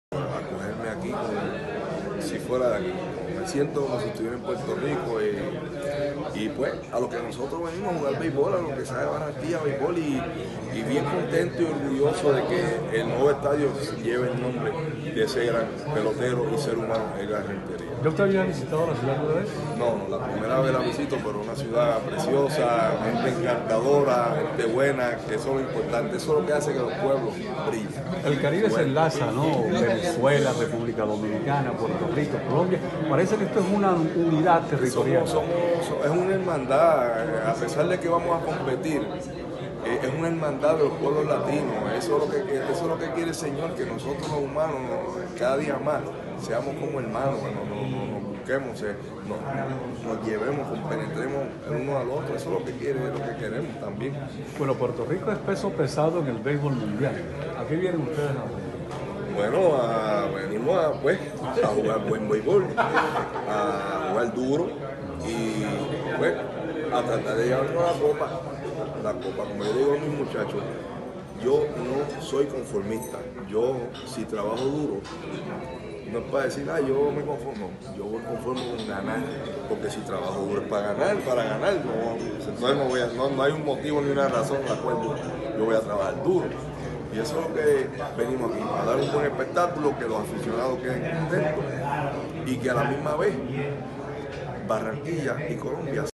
Juan “Igor” González, es el manager del equipo puertorriqueño de béisbol.
El experimentado manager del equipo de la ‘Isla del Encanto’, Puerto Rico lleva 16 años dirigiendo a peloteros de su país en diferentes equipos, tiene tres años de estar al comando de estos muchachos que buscan un lugar el la selección de béisbol nacional. Este es un equipo que no ha dejado de entrenar desde su arribo a Barranquilla, donde se sienten felices como en su casa, siempre destacando la alegría y amabilidad de los barranquilleros que los reciben con los brazos abiertos.Telocuentoya.com